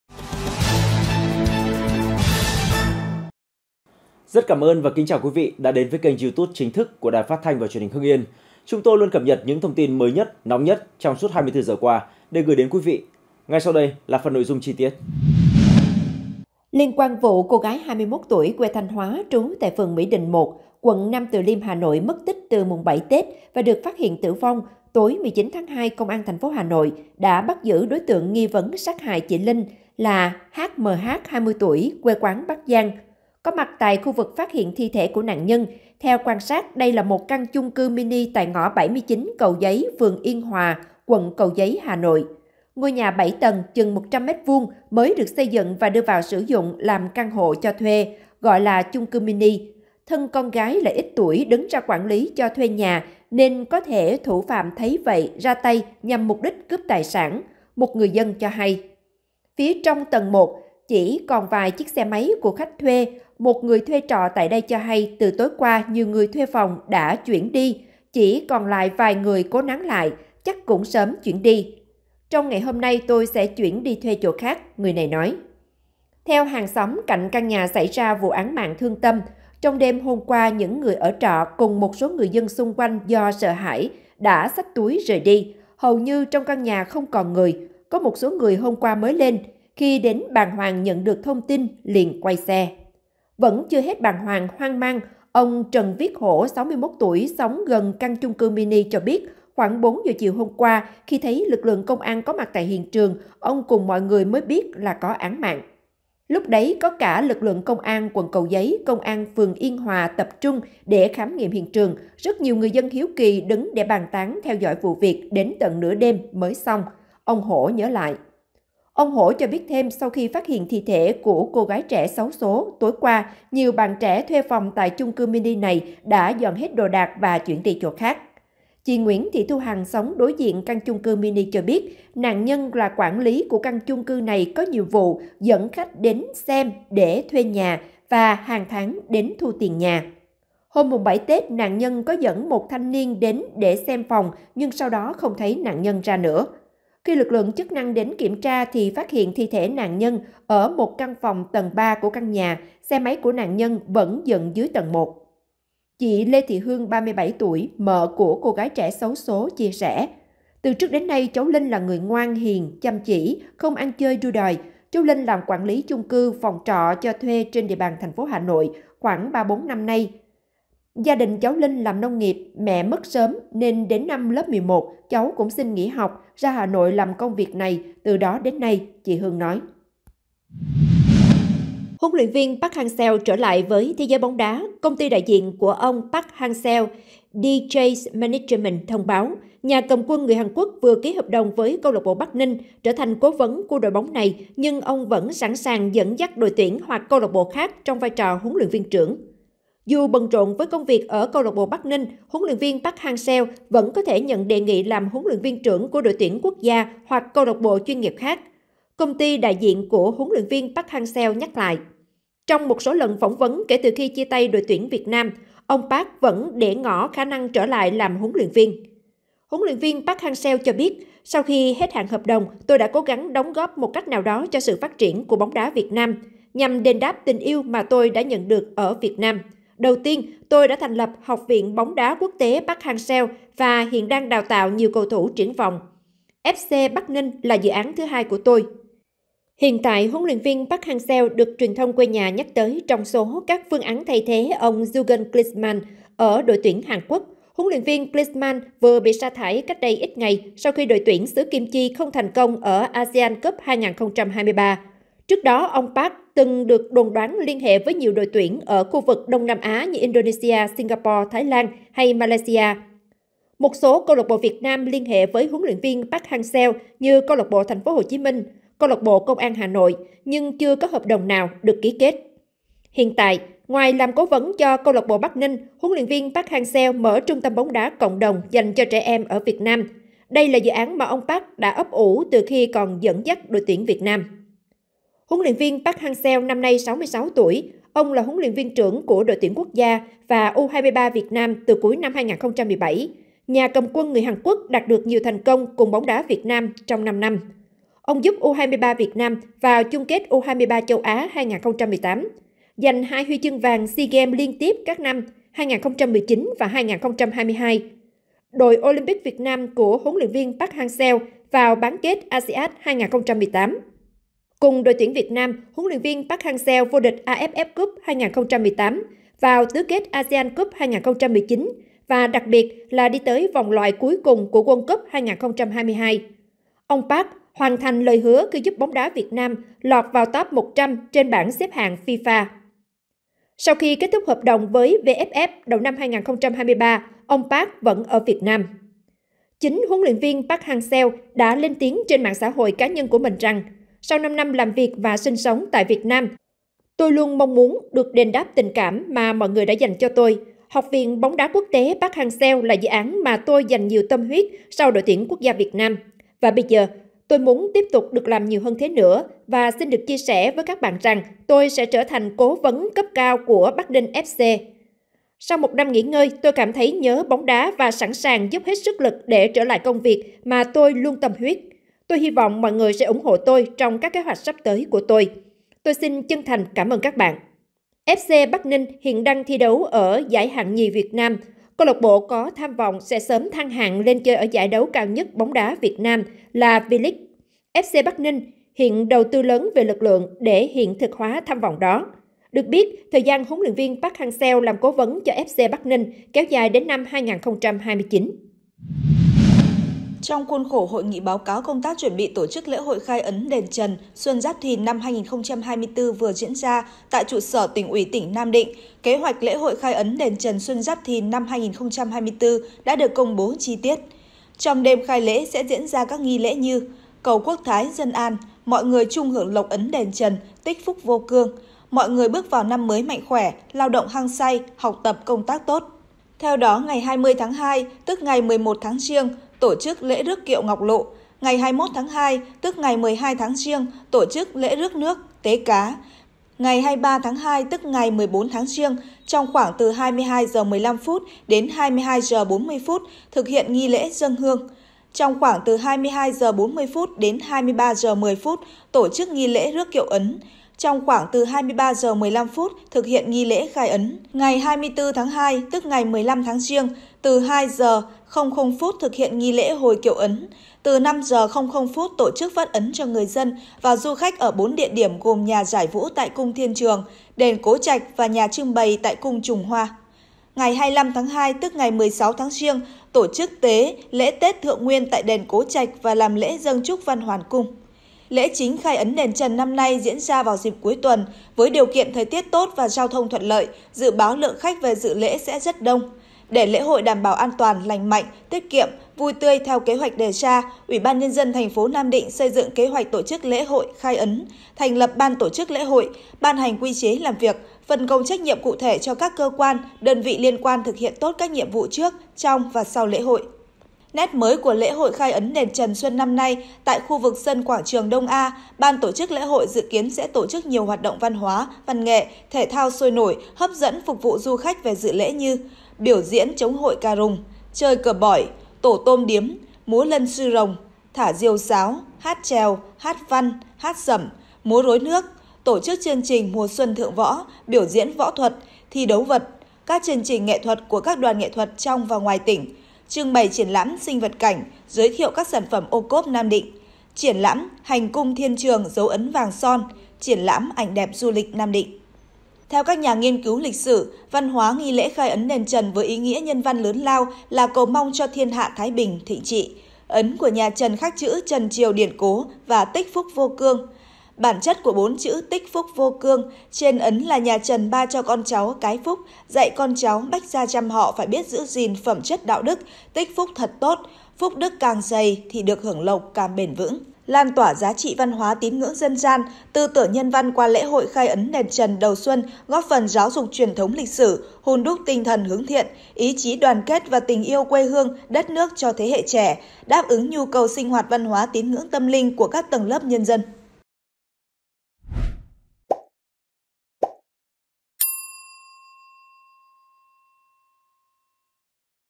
Diễn biến “lạ” vụ cô gái 21 tuổi bị sát hại: Nhiều người trong khu chung cư dọn đồ đi gấp trong đêm
Hưng Yên TV | Diễn biến “lạ” vụ cô gái 21 tuổi bị sát hại: Nhiều người trong khu chung cư dọn đồ đi gấp trong đêm
Liên quan vụ cô gái 21 tuổi, quê Thanh Hoá, trú tại phường Mỹ Đình 1, quận Nam Từ Liêm, Hà Nội mất tích từ mùng 7 Tết và được phát hiện tử vong, tối 19/2, Công an TP Hà Nội đã bắt giữ đối tượng nghi vấn sát hại chị Linh là H.M.H (20 tuổi, quê quán Bắc Giang).
Địa chỉ: 164 Nguyễn Văn Linh, thành phố Hưng Yên, tỉnh Hưng Yên
Tiktok: Truyền hình Hưng Yên
Facebook: Đài Phát thanh và Truyền hình Hưng Yên
Zalo: Đài PTTH Hưng Yên